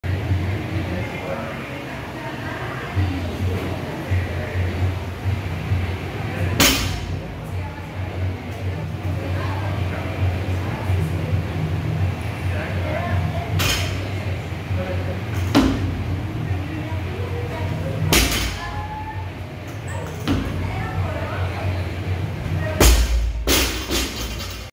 Snatch Complex Lift Off
Snatch Complex
1 Snatch Lift Off (despegue hasta abajo de la rodilla 3" pausa)
1 Snatch High Pull (bajando lento)
1 Power Snatch
1 Squat Snatch